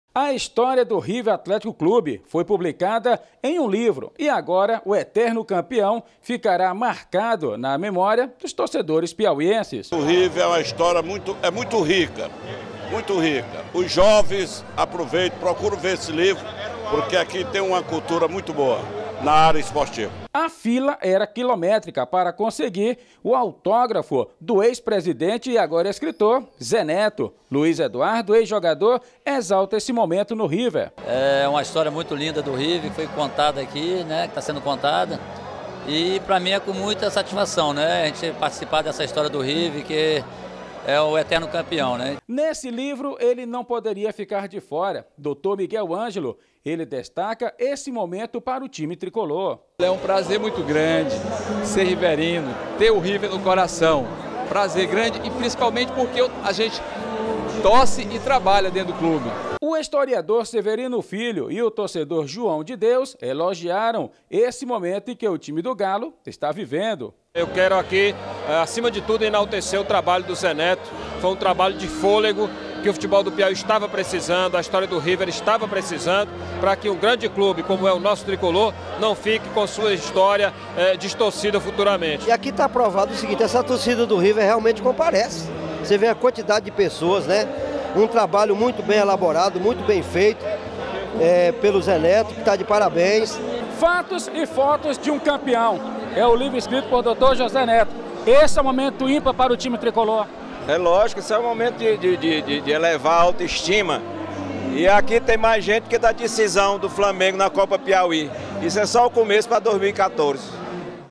LANÇADO LIVRO SOBRE A HISTÓRIA DO RIVER
A HISTÓRIA DO GLORIOSO RIVER ATLÉTICO CLUBE VIROU LIVRO QUE FOI LANÇADO NA NOITE DE ONTEM.